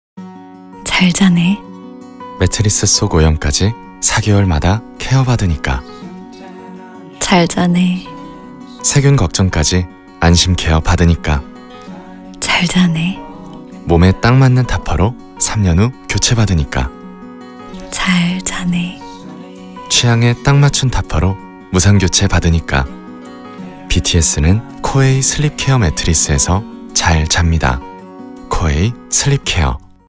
[COWAY x BTS] 코웨이 슬립케어 매트리스_뷔/슈가/정국/RM편(30)
[BTS의 꿀잠비결]

매트리스 속 오염까지 4개월 마다 케어 받으니까
세균 걱정까지 안심케어 받으니까
몸에 딱 맞는 탑퍼로 3년 후 교체 받으니까
취향에 딱 맞춘 탑퍼로 무상교체 받으니까

BTS는 코웨이 슬립케어 매트리스에서 잘 잡니다

BTS is getting a good night’s sleep on their Coway SLEEP CARE Mattress. 💜
Why don’t you join in on the fun to watch coway Sleepcare Mattress featuring BTS.